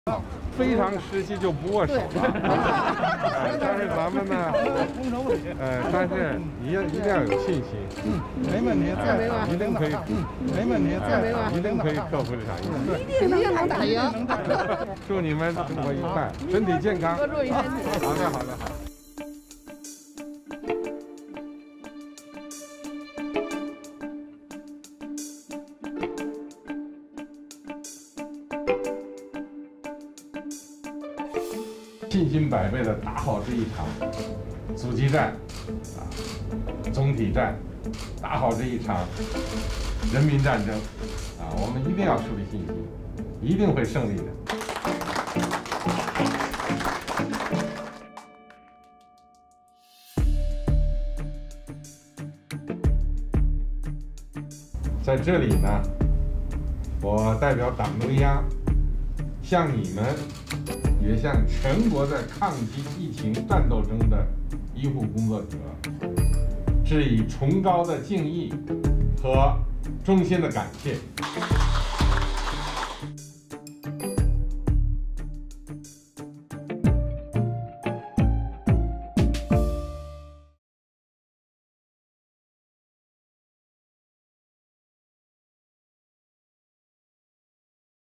Masqué, Xi appelle à des mesures plus fortes contre le coronavirus
Tandis que le bilan du coronavirus s'alourdit en Chine, passant à plus de 900 morts, le président chinois Xi Jinping s'est rendu au contact de la population et des des hôpitaux de Pékin où sont soignées des victimes. Il a tenu, masqué, à encourager le personnel médical et rassurer sur la situation, tout en appelant à des mesures plus fortes contre l'épidémie.